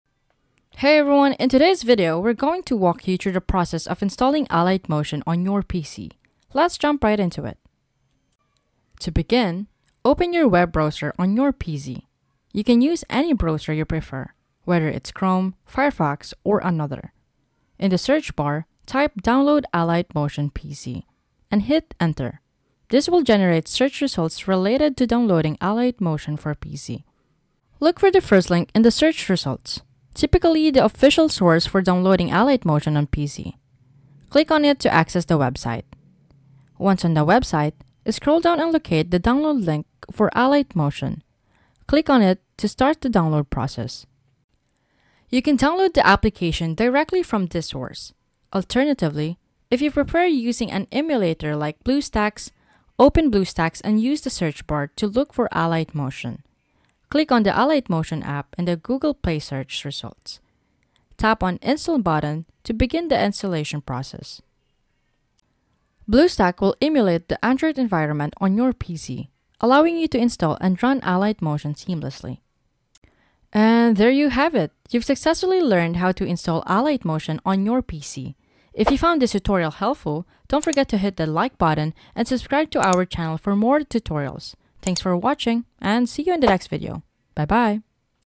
How to Download Alight Motion on Laptop || Install Alight Motion on Windows
How to Download Alight Motion on Laptop

In this video I'll show you how to Download Alight Motion on Windows Laptop. The method is very simple and clearly described in the video. Follow all of the steps & Download Alight Motion on Laptop and PC

How to Download Alight Motion on Laptop and PC